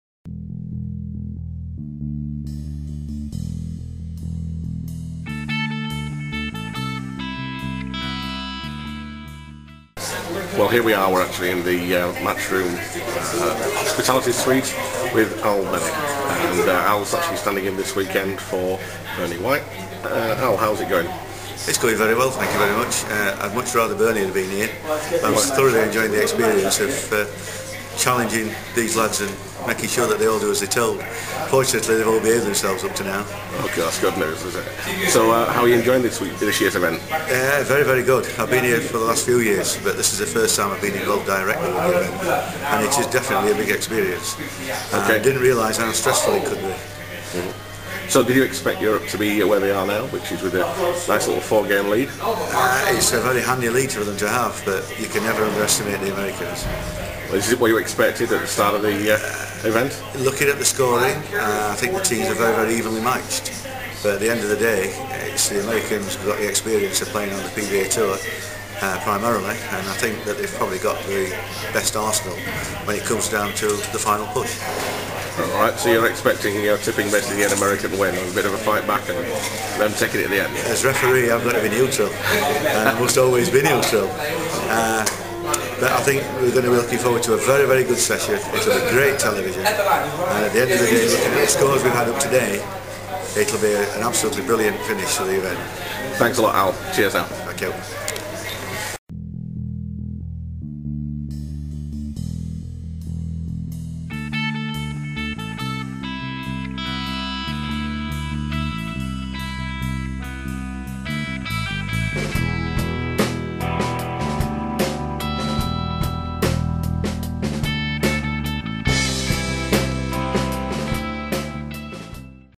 Weber Cup - Match Referee Al Bennett Interviewed
YBTBA's Al Bennett standing in for an Ill Bernie White talks to TT's John in his first video podcast

Get well soon Bernie from us all at talk tenpin